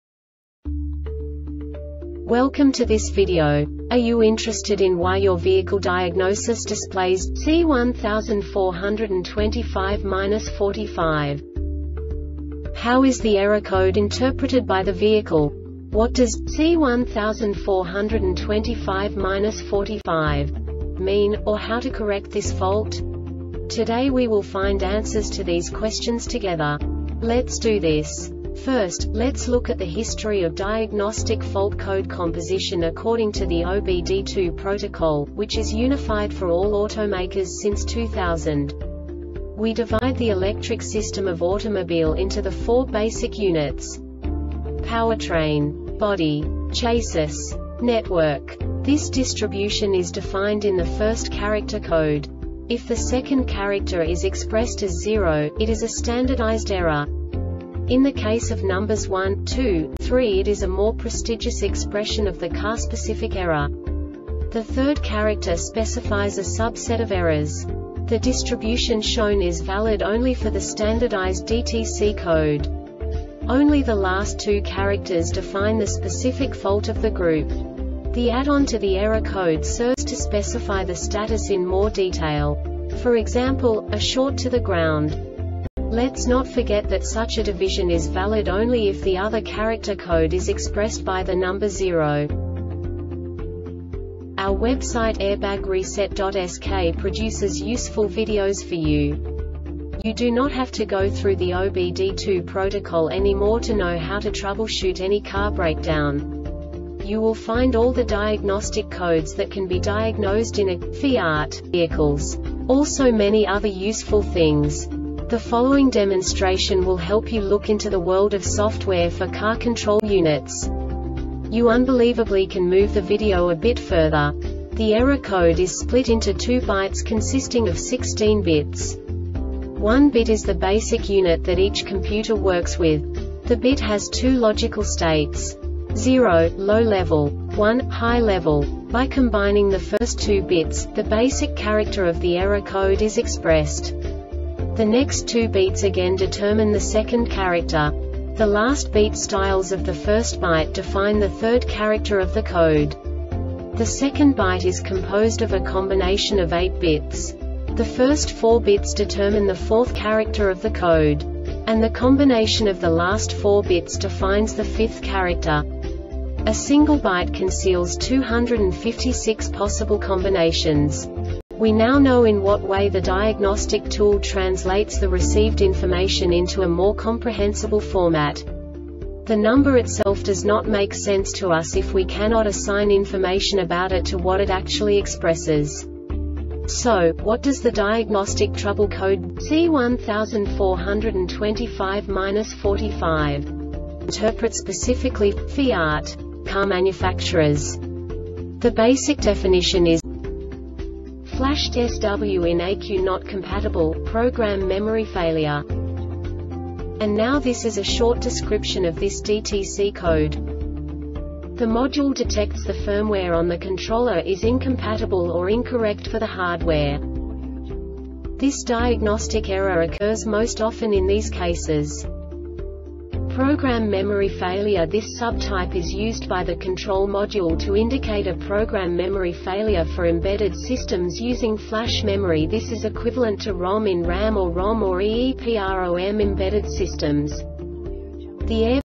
DTC Fiat C1425-45 Short Explanation
Contents:
0:21 Basic DTC analysis according to OBD2 protocol standard.
1:48 Insight into programming
2:58 A diagnostically understandable description of the Diagnostic Trouble Code
3:05 Basic error definition

Make:
FIAT
Code:
C1425-45
Definition:
Flashed Sw In Ecu Not Compatible - Program Memory Failure
Description:
The module detects the firmware on the controller is incompatible or incorrect for the hardware.
Failure Type:
Program Memory Failure
This sub type is used by the control module to indicate a program memory failure for embedded systems using FLASH memory. This is equivalent to ROM in RAM or ROM or EEPROM embedded systems.